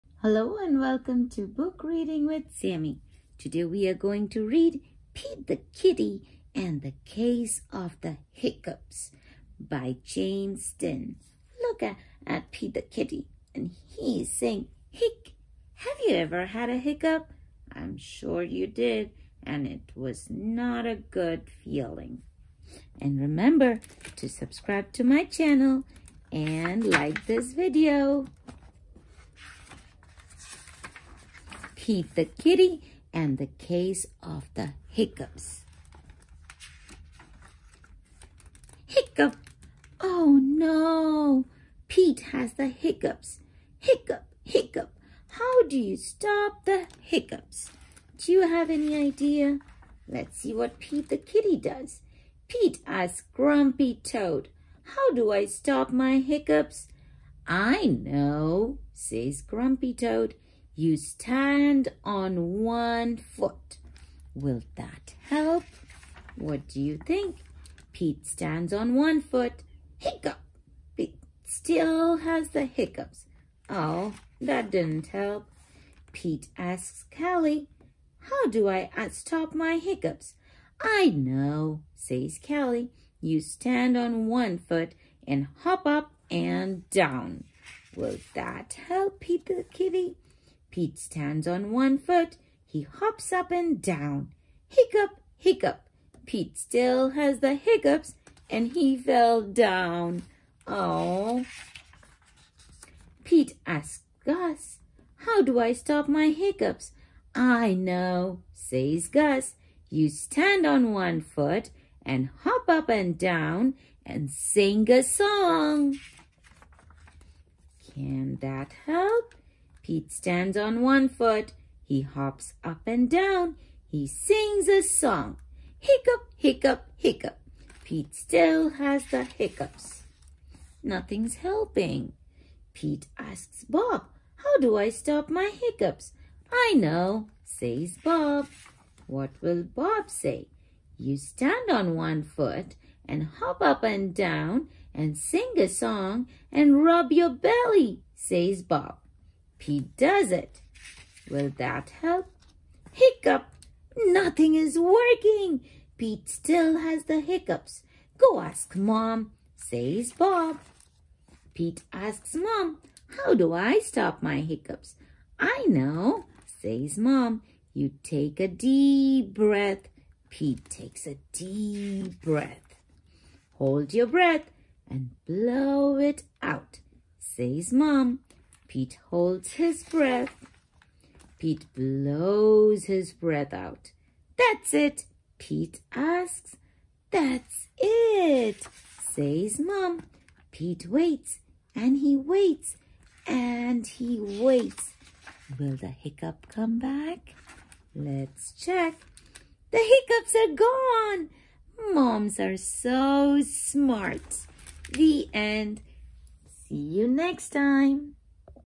Read Aloud Book- Pete the Kitty and The Case of The Hiccups
Pete the Kitty and The Case of The Hiccups
by James Dean
Pete gets a case of the hiccups, and he doesn’t know what to do! He decides to ask his wisest friends for help, like Grumpy Toad, Callie, and Bob. But Pete is still stuck with the hiccups! Will Pete the Kitty ever get rid of his terrible hiccups?

Beginning readers will laugh and easily relate to Pete the Kitty and his search for the perfect remedy for his hiccups. My First I Can Read books are perfect for shared reading with a child.
Description from Goodreads